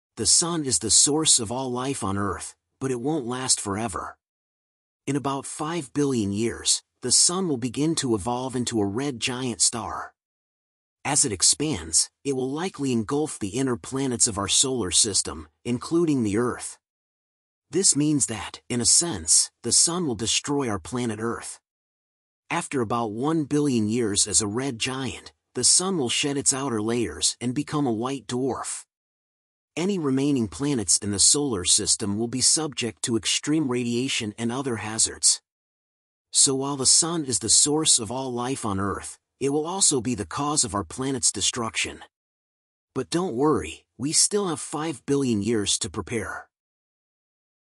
The Sun will Destroy Earth
The Sun is the source of all life on Earth, but it won't last forever.

In about 5 billion years, the Sun will begin to evolve into a red giant star. As it expands, it will likely engulf the inner planets of our solar system, including the Earth.

This means that, in a sense, the Sun will destroy our planet Earth.

After about 1 billion years as a red giant, the Sun will shed its outer layers and become a white dwarf. Any remaining planets in the solar system will be subject to extreme radiation and other hazards.

So while the Sun is the source of all life on Earth, it will also be the cause of our planet's destruction. But don't worry, we still have 5 billion years to prepare.

Attribution

Thanks to :
European Southern Observatory (ESO